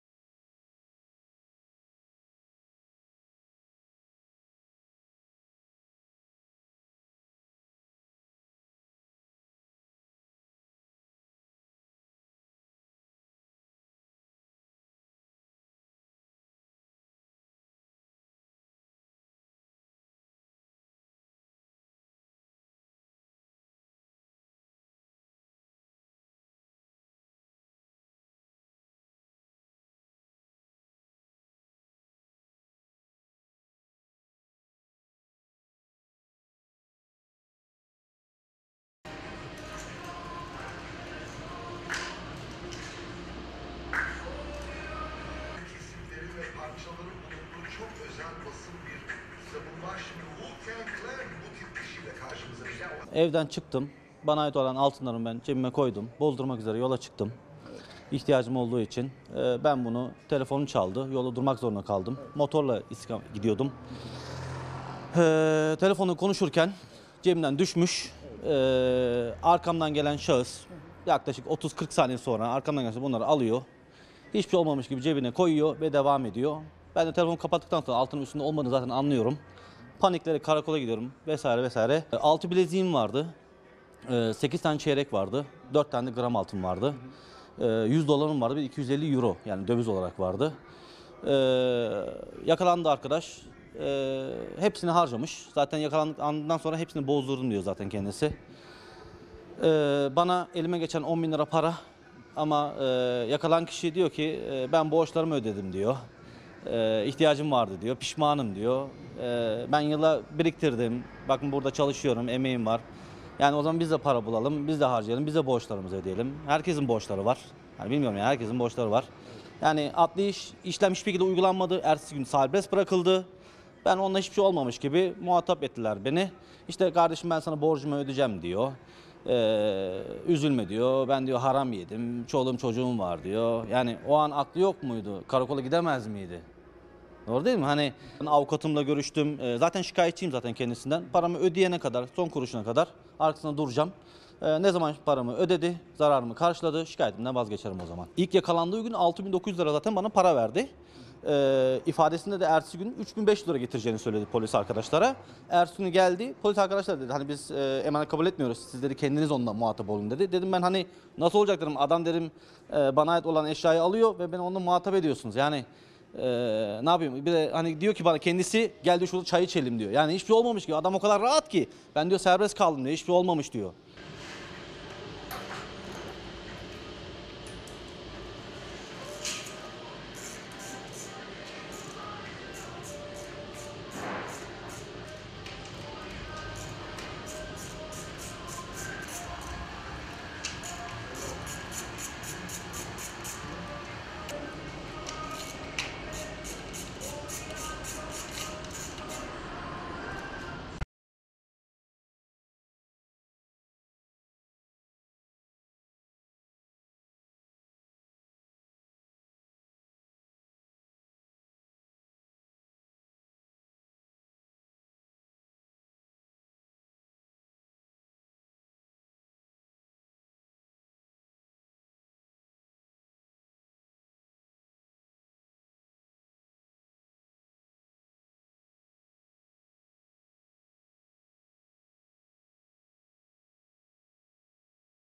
"Pardon altınlarınızı satıp yedim, param olursa ödeyeceğim"
Denizli’de yeni evli çiftin bozdurmak için kuyumcuya götürdüğü altınların düştüğünü görmesine rağmen sahibine vermek yerine cebine atan kişi tespit edildi. Altın satışından eline geçen 48 bin liranın 38 bin lirasını harcamasına rağmen serbest kalan kişi, geriye kalan miktarı eline para geçerse ödeyeceğini belirtti.